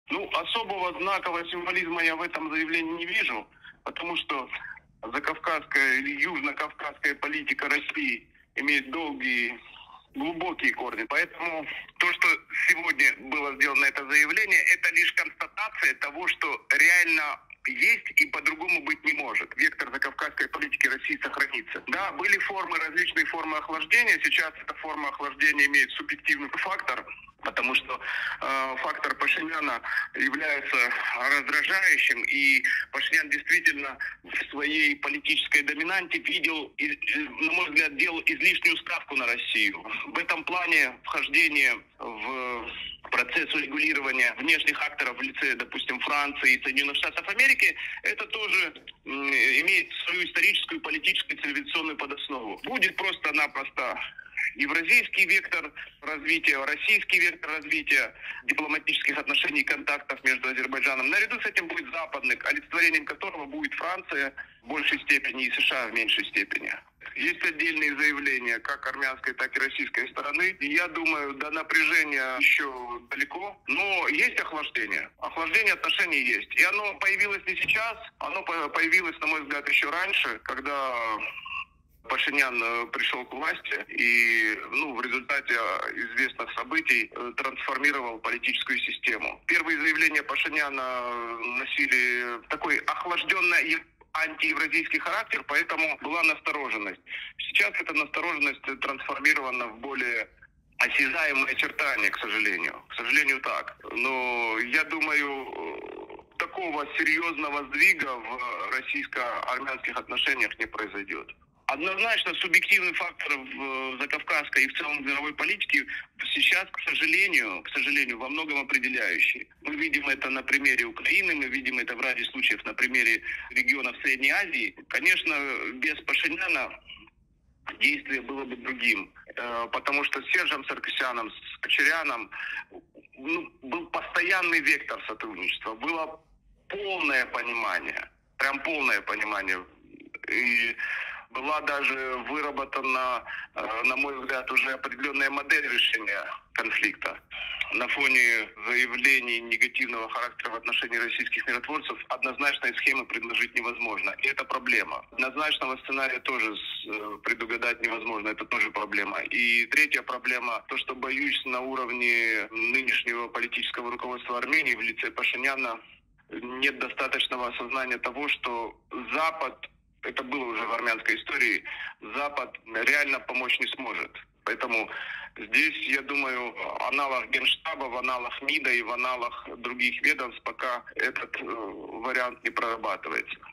Запад реально помочь не сможет Армении, что было видно в армянской истории - Артур Атаев
Политолог Артур Атаев прокомментировал Alpha News недавние заявления  Кремля и МИД РФ, что Москва, несмотря ни на что, все равно готова к продолжению диалога с Ереваном и Баку.

«Особого символизма я в этом заявлении не вижу, потому что закавказская политика России имеет глубокие корни. Сделанные заявления — лишь констатация того, что реально есть и по-другому быть не может. Вектор закавказской политики России сохранится», - отметил Атаев.

Подробнее на видео